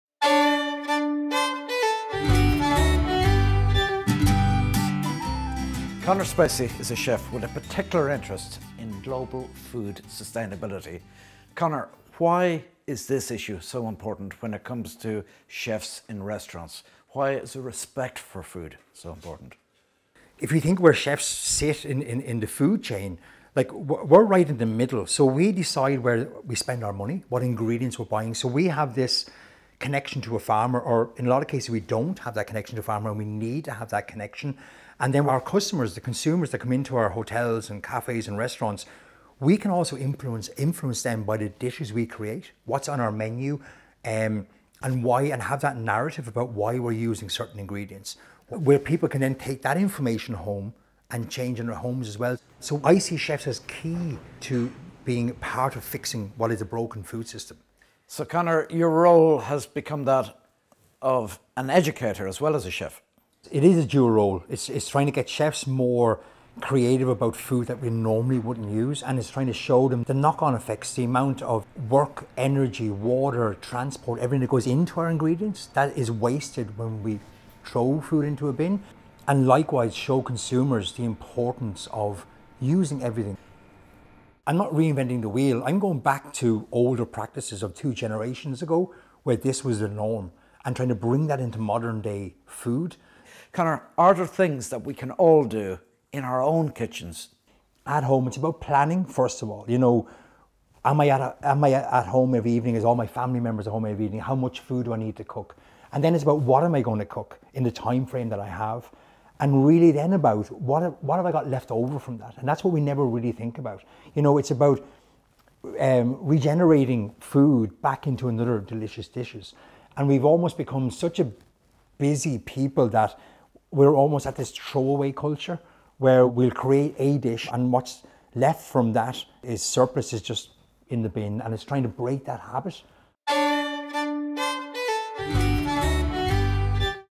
Diarmuid Gavin与厨师、零粮食浪费倡导者Conor Spacey的对话
Conor Spacey 是一位特别关注全球粮食可持续发展的厨师